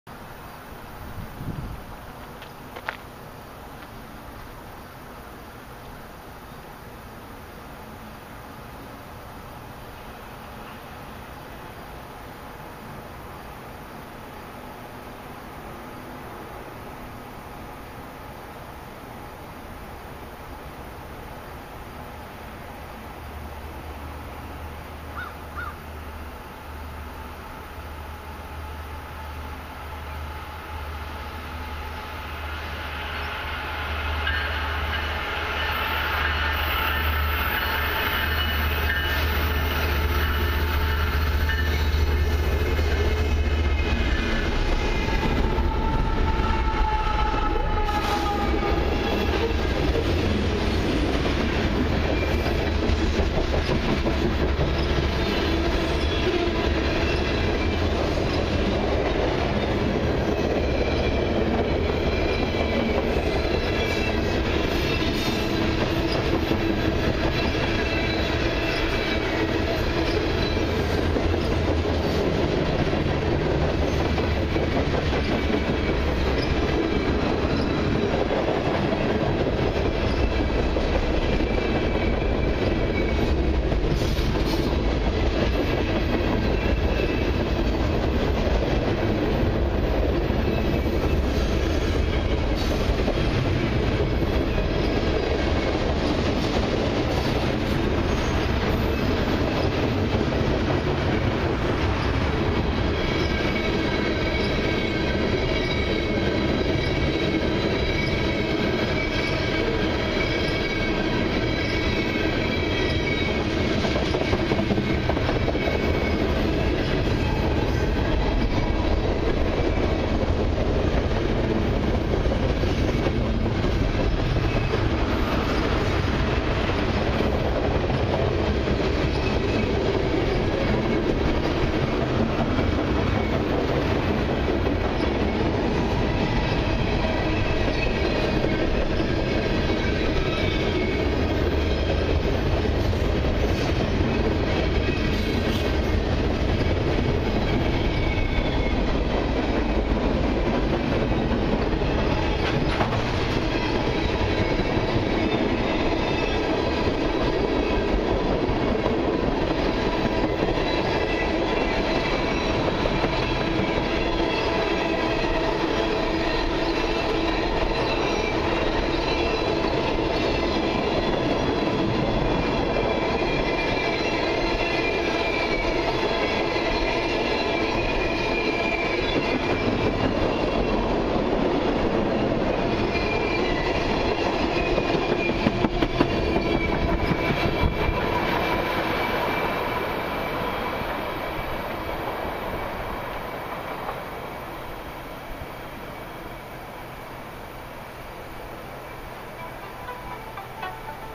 CN 302 w/ CN Engines 2308, 2298 & 3933 (ex CREX) lead this long 454 axle, EB Mixed Freight train by
.Georgetown GO/VIA Station at the MP of 23 on the CN Halton Sub.

Here's my 7th catch from back on Sept 24th.

With CN 3933 being ex CREX 1311.

video taken: Saturday September 24th 2022 at 3:35pm